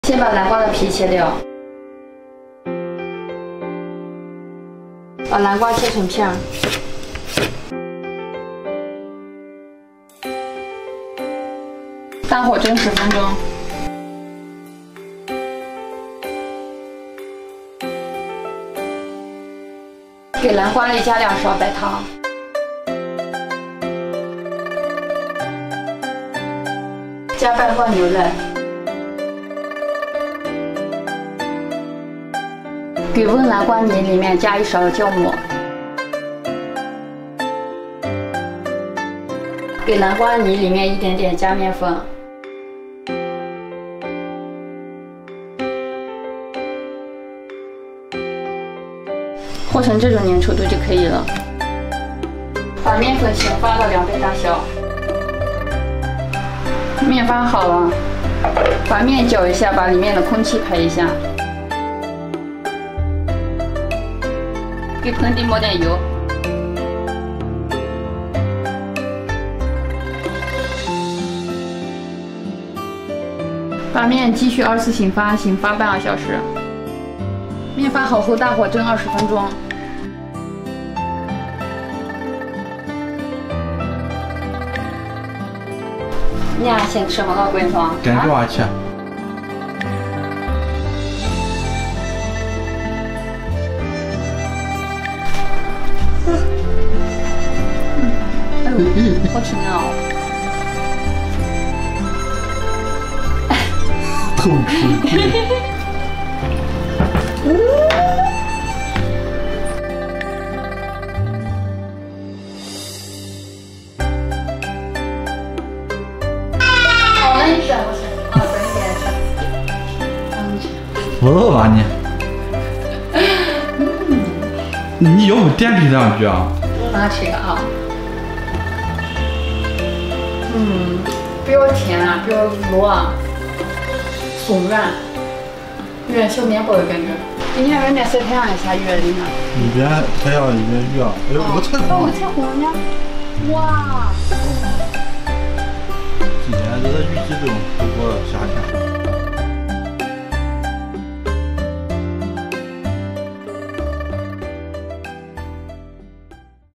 【小胖在西北】南瓜发糕超美味的做法，不用一滴水，香甜松软，比蛋糕都好吃
南瓜发糕

原材料：南瓜，面粉，白糖，酵母

注意事项：
1.加酵母的时候南瓜泥有37度左右最好
2.加点白糖帮助发酵，节省发酵时间
3.盆底抹点油这样做出来的发糕就可以倒出来了
4.水烧开大火蒸二十分钟以上 "我们的目的是分享美味食品的制作过程，所有食材的获取和使用都符合道德和法律原则。"